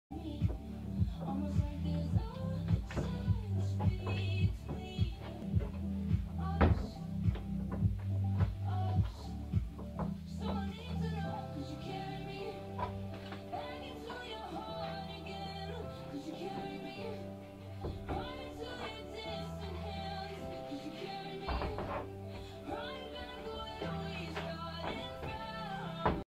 grüne Route
Habe mir im Garage eine kleine Boulderwand gebaut